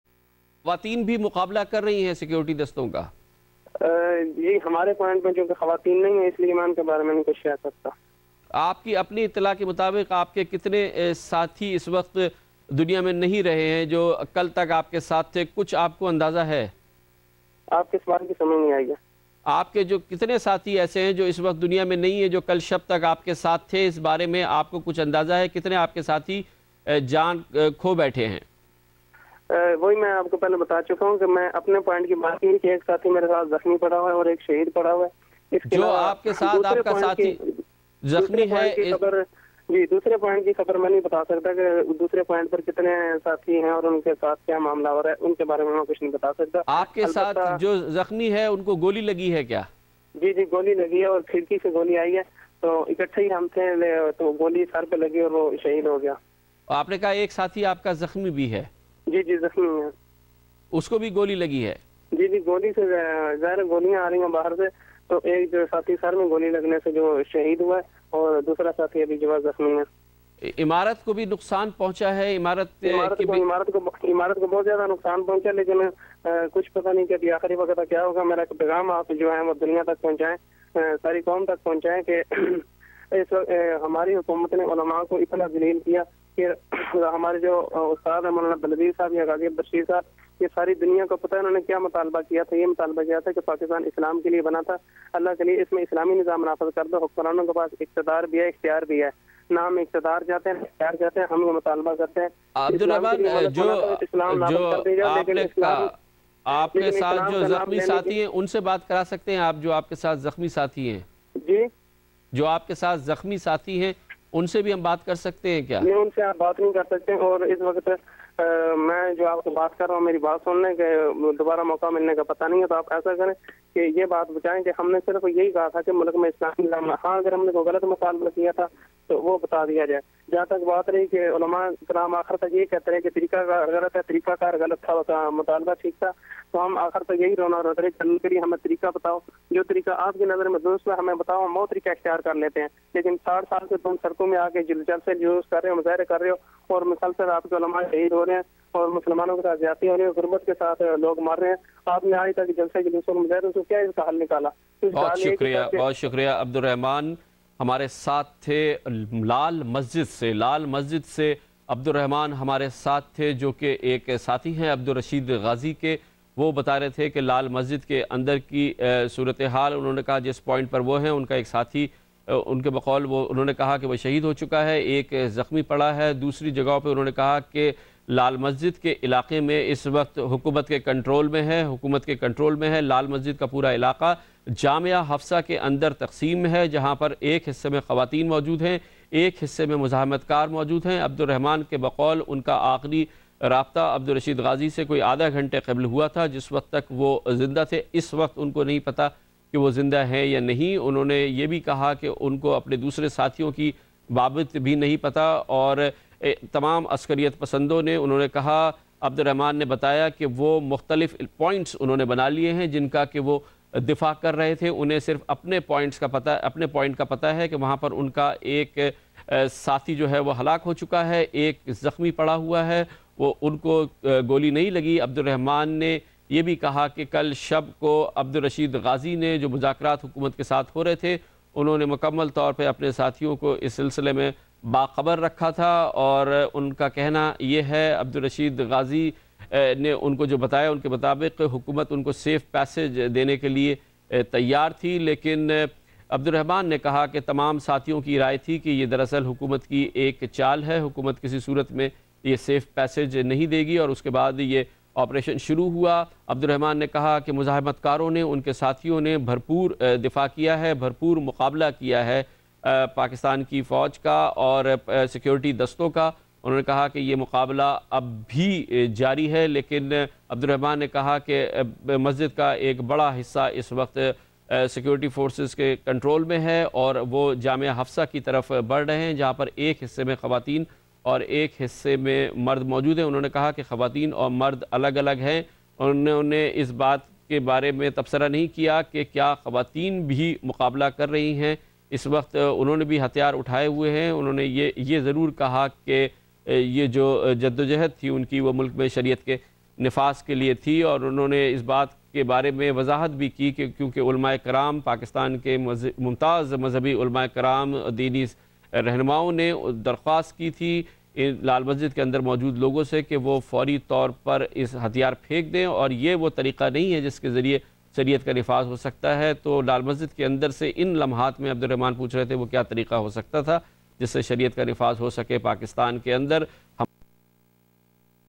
Lal Masjid Member Abdur Rahman Live from inside. Part 2/2
This is a live interview with one of Lal Masjid Members, Abdur Rahman, fighting "Brave" Pak Army.